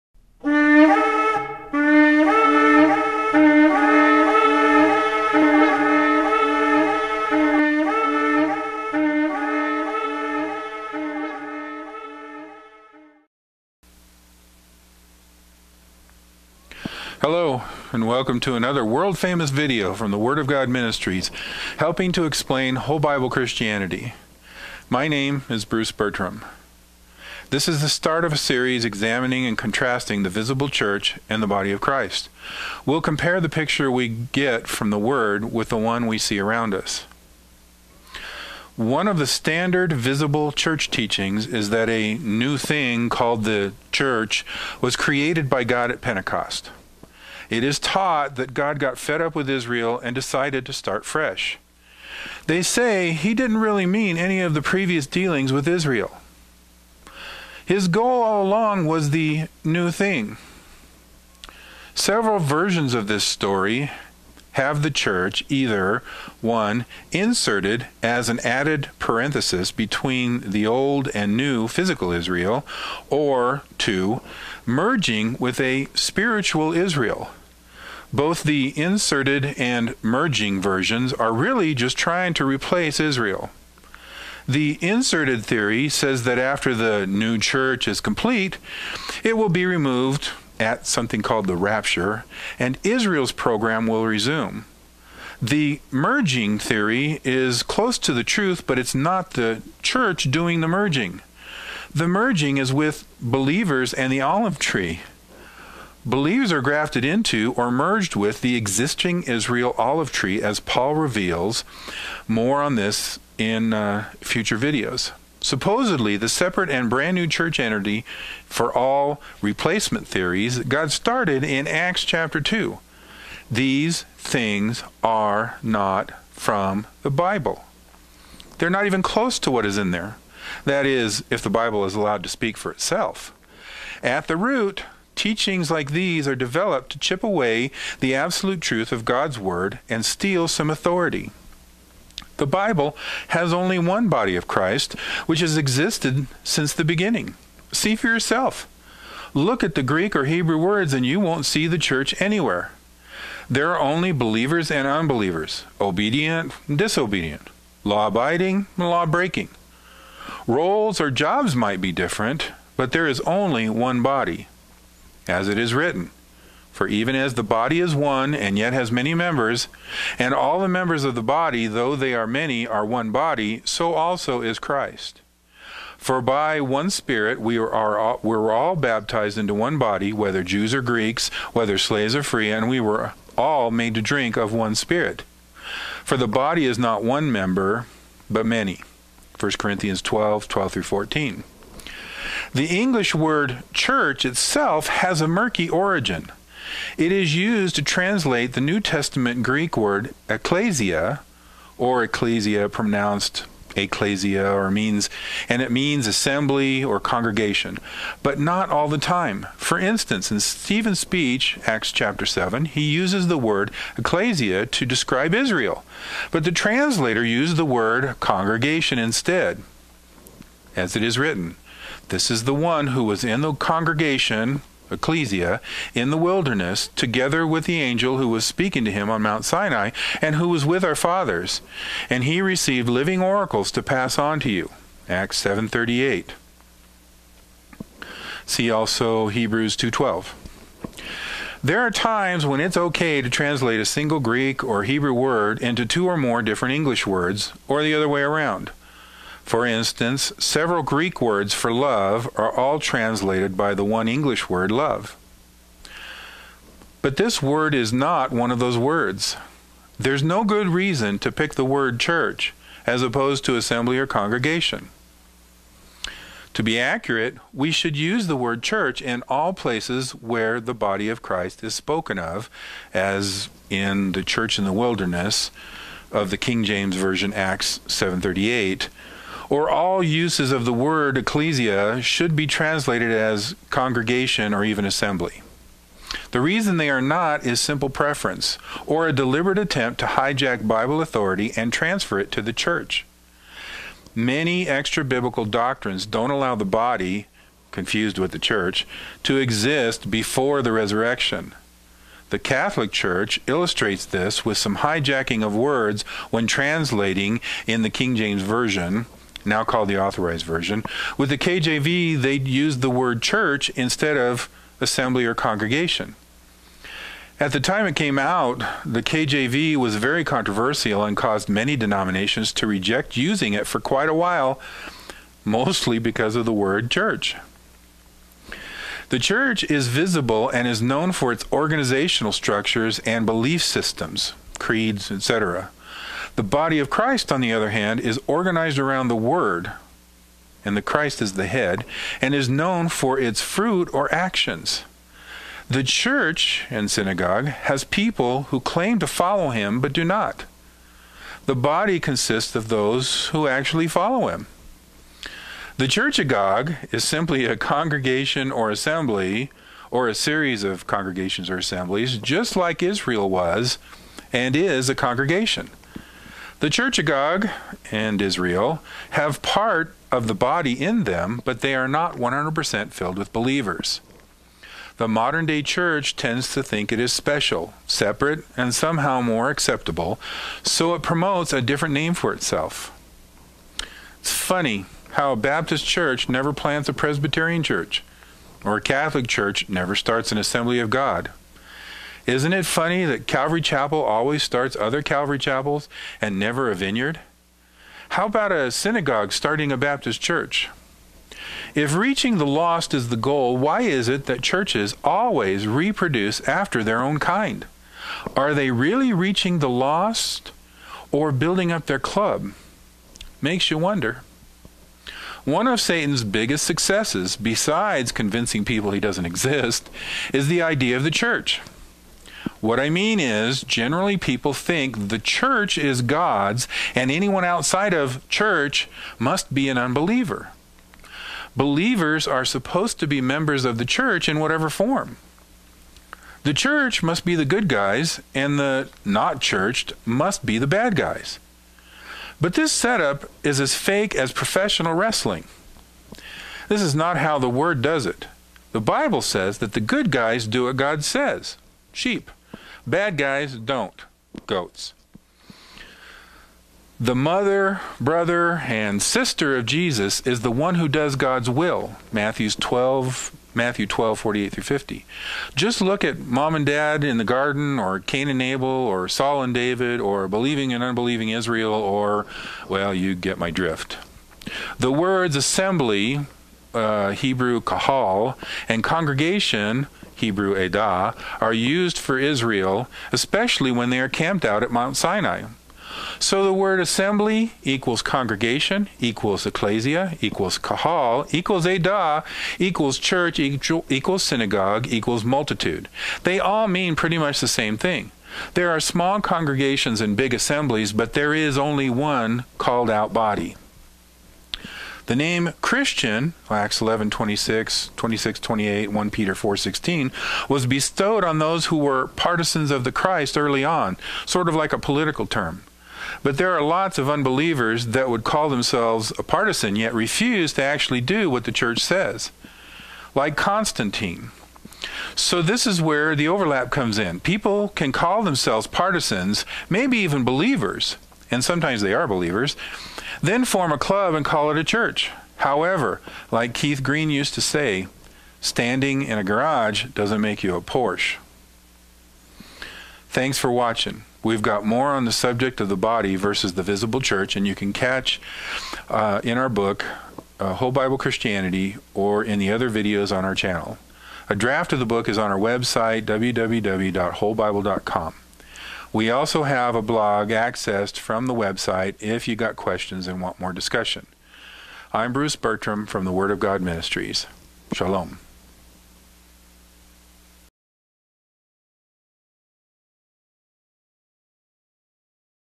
A Whole Body: A New Thing?
Available also in Kindle and Audiobook formats!

Bruce Bertram from The Word of God Ministries presents the whole Bible Christian view of the Body in this and following videos. According to modern church teachings a "new thing" (called the church) was created by God at Pentecost. But was it? A new thing has certainly been created, but not by God. The Body of Christ as been around for far longer than Acts chapter 2. Biblically, the book of Acts tells us of the continuation of the Body, not the beginning.